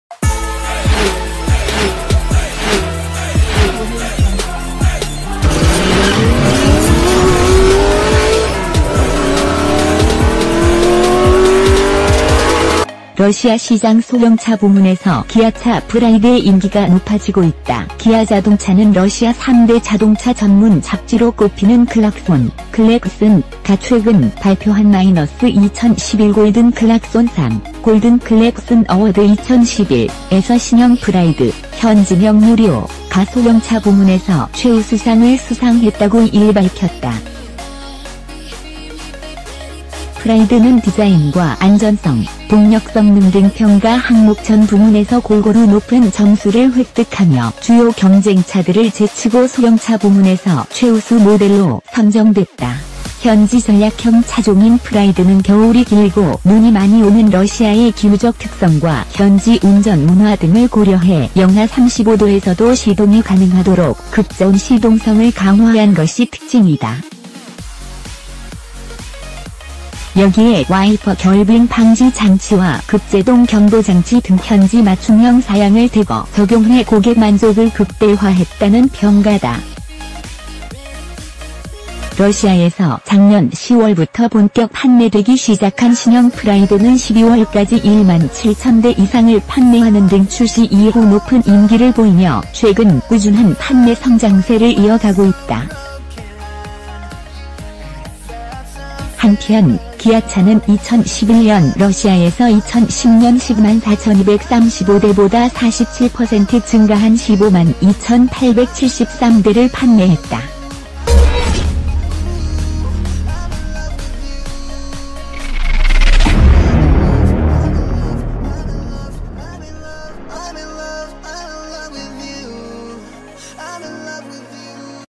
기아차 프라이드, “러시아서 맞춤형 서비스 통했다” |카24/7
러시아 시장 소형차 부문에서 기아차 프라이드의 인기가 높아지고 있다.

기아자동차는 러시아 3대 자동차 전문 잡지로 꼽히는 ‘클락손(Klaxon)’誌가 최근 발표한 ‘2011 골든 클락손 상(Golden Klaxon Award 2011)’에서 신형 프라이드(현지명 ‘뉴 리오’)가 소형차 부문에서 최우수상을 수상했다고 2일 밝혔다.

골든 클락손 상은 ▲디자인 ▲품질 ▲안전성 ▲연비 ▲동력성능 ▲신기술 등 총 25개 항목에 대한 자동차 전문기자들의 평가를 통해 총 12개 차급별 1위를 가린다.

프라이드는 디자인과 안전성, 동력성능 등 평가 항목 전 부문에서 골고루 높은 점수를 획득하며 주요 경쟁차들을 제치고 소형차 부문에서 최우수 모델로 선정됐다.

현지 전략형 차종인 프라이드는 겨울이 길고 눈이 많이 오는 러시아의 기후적 특성과 현지 운전 문화 등을 고려해 영하 35도에서도 시동이 가능하도록 극저온 시동성을 강화한 것이 특징이다.

여기에 와이퍼 결빙 방지 장치와 급제동 경보장치 등 현지 맞춤형 사양을 대거 적용해 고객 만족을 극대화했다는 평가다.

러시아에서 작년 10월부터 본격 판매되기 시작한 신형 프라이드는 12월까지 1만7000대 이상을 판매하는 등 출시 이후 높은 인기를 보이며 최근 꾸준한 판매 성장세를 이어 가고 있다.

한편, 기아차는 2011년 러시아에서 2010년 10만4235대보다 47% 증가한 15만2873대를 판매했다.